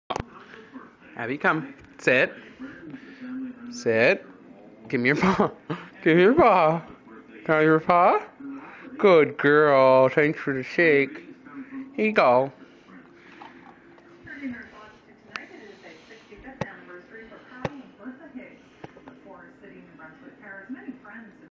Abby learning tricks. english mastiff puppy
3 months old, learning new tricks